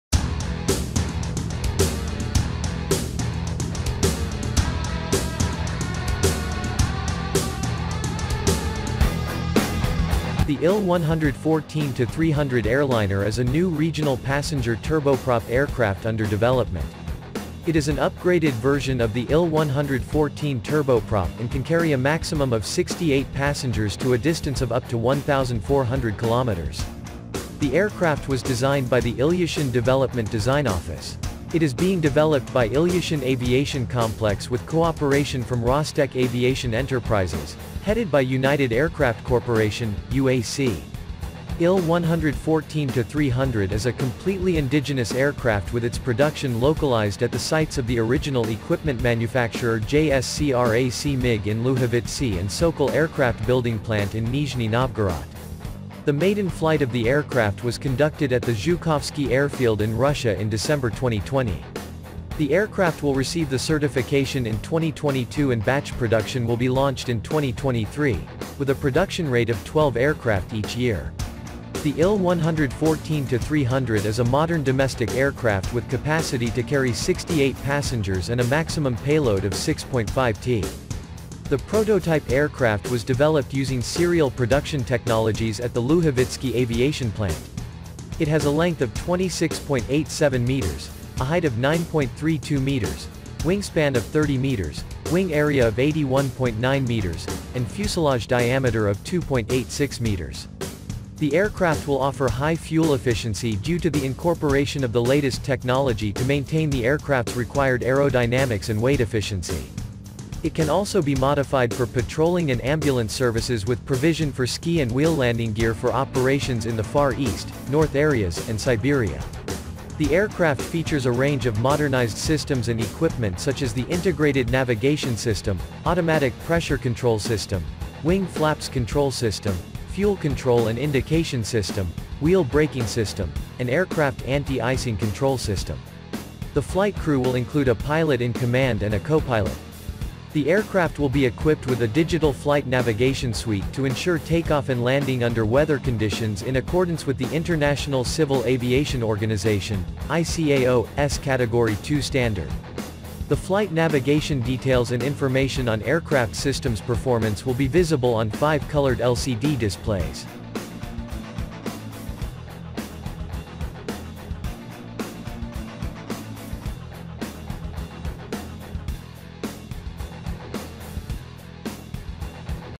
RUSSIA’S NEXT DOMESTIC AIRLINER Il 114 Will Be Ready In A Year
The IL-114-300 airliner is a new regional passenger turboprop aircraft under development. It is an upgraded version of the IL-114 turboprop and can carry a maximum of 68 passengers to a distance of up to 1,400km.

The aircraft was designed by the Ilyushin Development Design Office. It is being developed by Ilyushin Aviation Complex with cooperation from Rostec aviation enterprises, headed by United Aircraft Corporation (UAC).